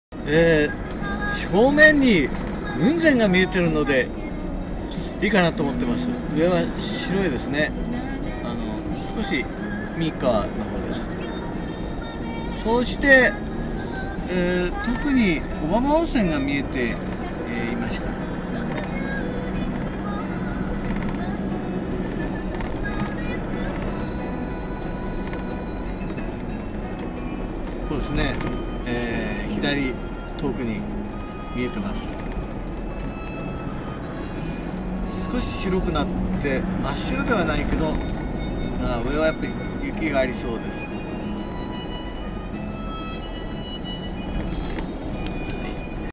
島原半島の西海岸を走る 正面に雲仙岳が (2016/1/14)
写真は、ブログ「キャンピングカーで放浪の旅Ⅱ」にあります。
下をクリックすると、その記事に。